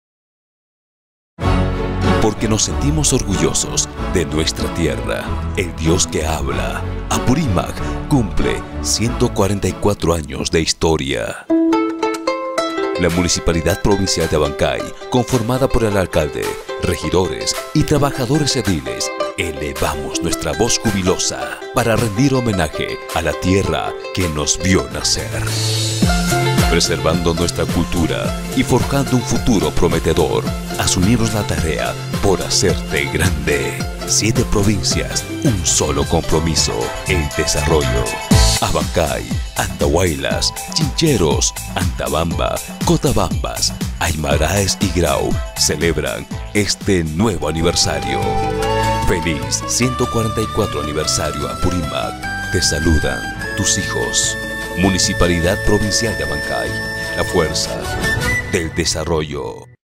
FELIZ 144° ANIVERSARIO APURÍMAC
En tus 144° Aniversario  mi Apurímac te saludad tus hijos de la Municipalidad Provincial de Abancay.